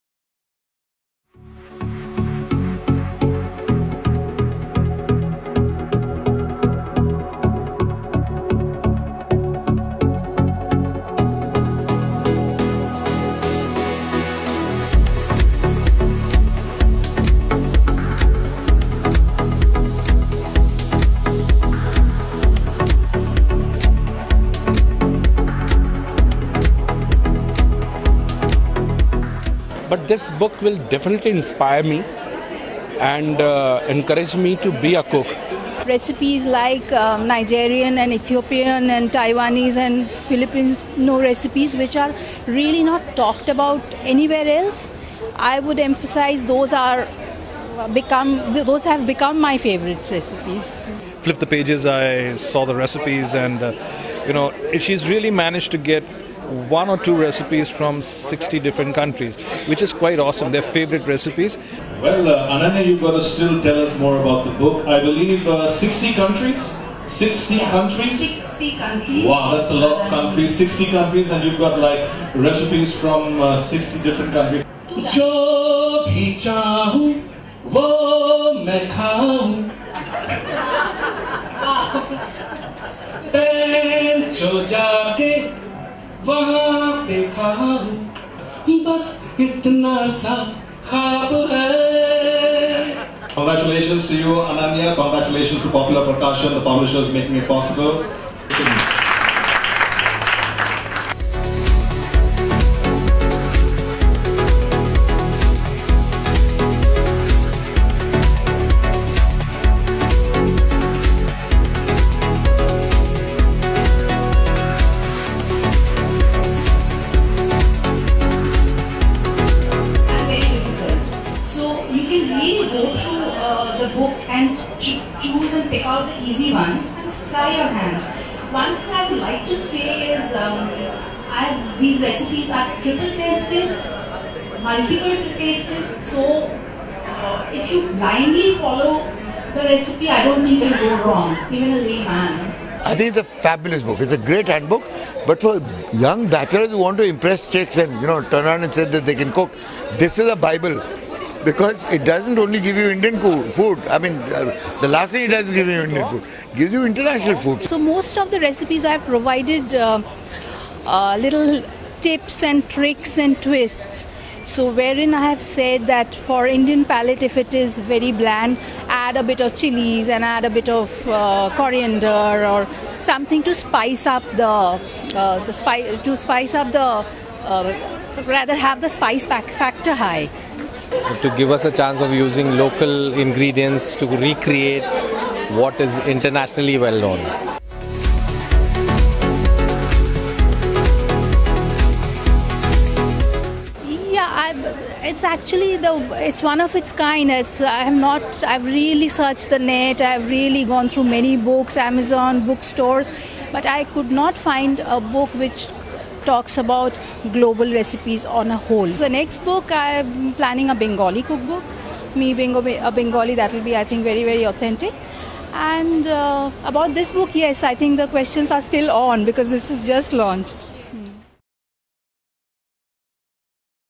Ananya Banerjee's first ever Book Launch 'Planet Gastronomy'
Ananya Banerjee's first ever Book Launch Planet Gastronomy held @ Crossword, Kemps Corner, Mumbai